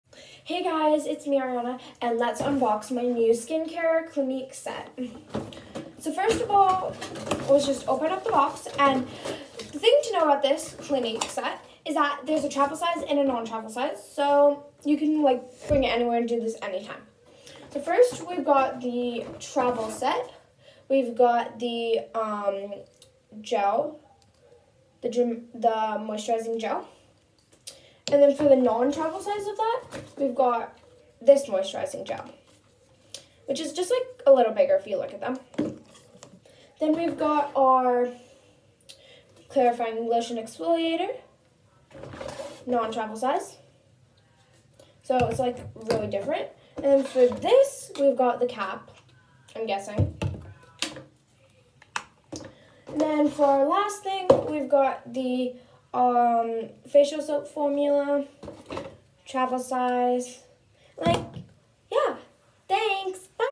Unbox My Skincare with Me
Unbox this fun Clinique skincare set with me